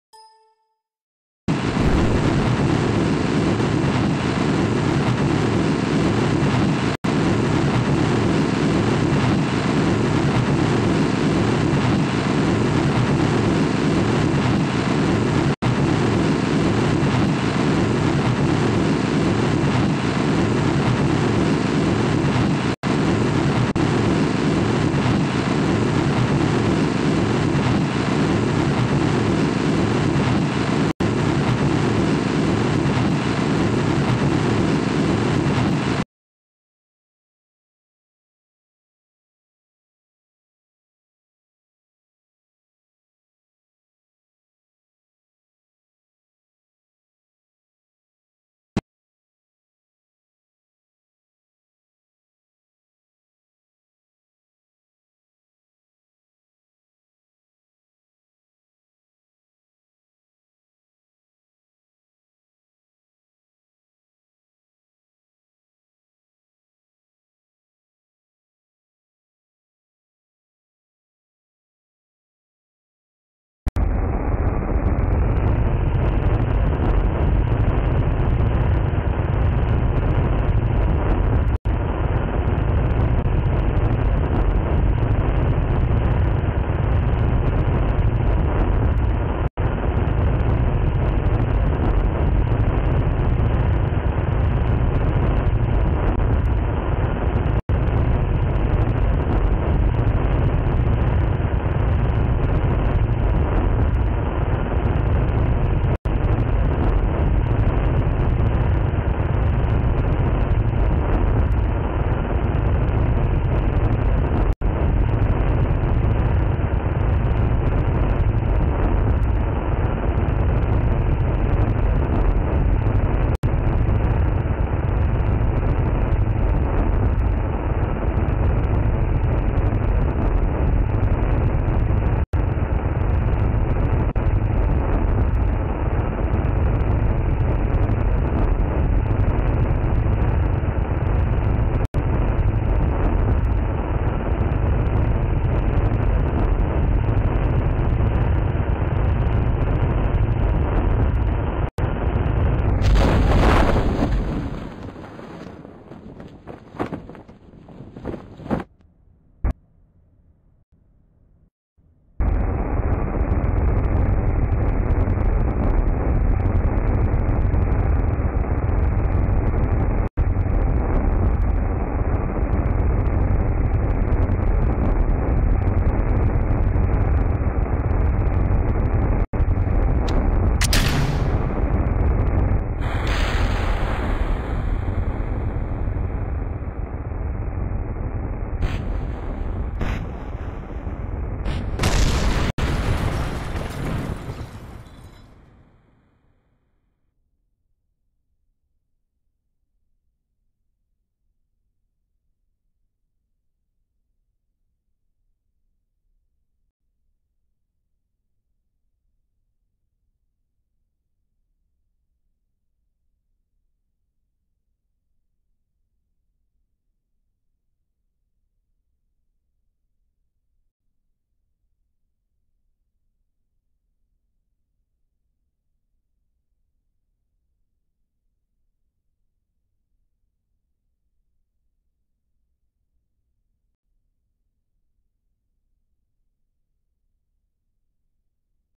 Duna Explorer landing
Landing of the Duna Explorer. After dumping in the interplanetary transfer stage and 102.6 tons of fuel, the 27.8 ton lander begins its decent. When it lands, the rover weighs 9.8 tons.
This is part 2 of the mission
Photos from the transfer stage